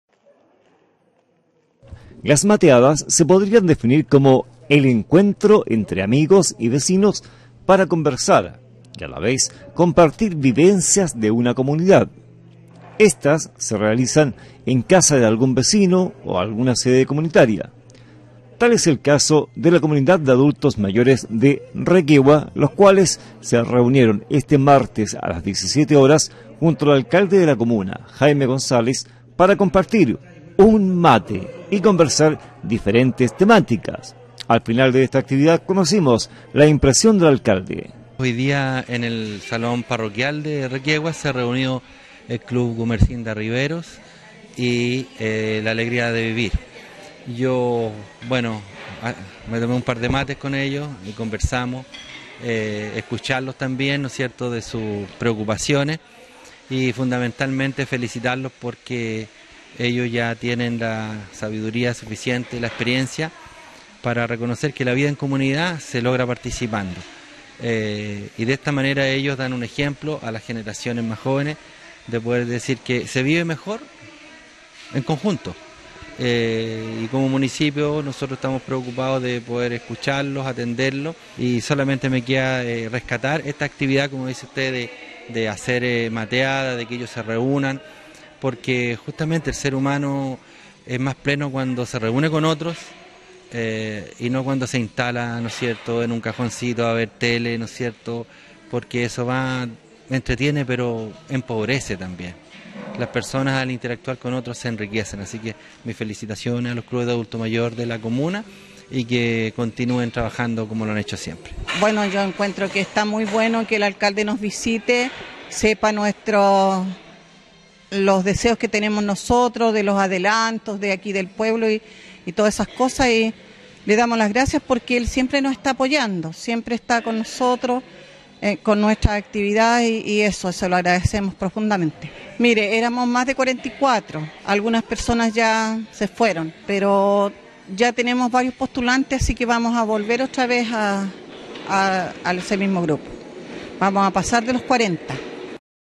Las mateadas: siempre vigentes entre clubes de adultos mayores
Las mateadas podrían definirse como "el encuentro entre amigos y vecinos para conversar y a la vez compartir vivencias de una comunidad".

Estas se realizan en casa de un vecino o casa comunitaria, como ocurrió en la sede del club de adultos mayores de Requegua los cuales se reunieron este martes a las 17:00 horas junto al alcalde de la comuna Jaime González para compartir una tertulia.